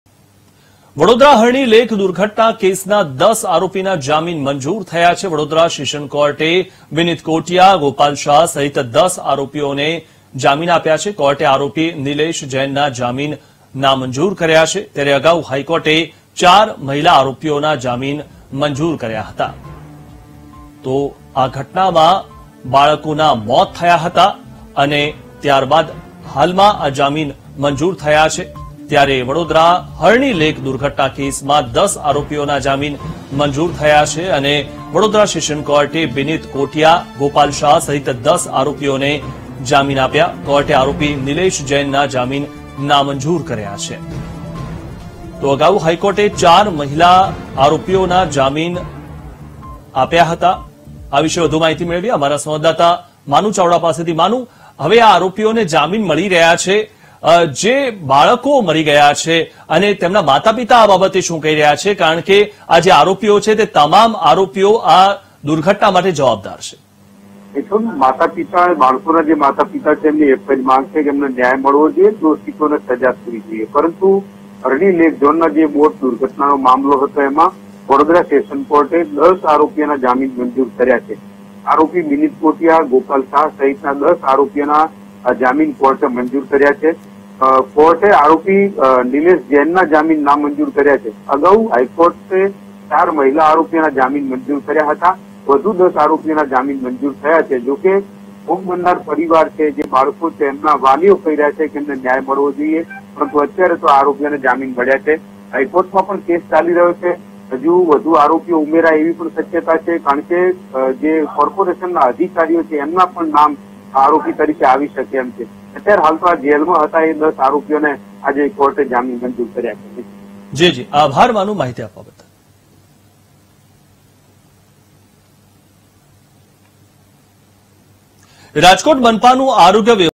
Vadodara News: વડોદરા સેશન્સ કોર્ટે બિનિત કોટિયા, ગોપાલ શાહ સહિત 10 આરોપીઓને આપ્યા જામીન
Vadodara News: વડોદરા સેશન્સ કોર્ટે બિનિત કોટિયા, ગોપાલ શાહ સહિત 10 આરોપીઓને આપ્યા જામીન I VTV GUJARATI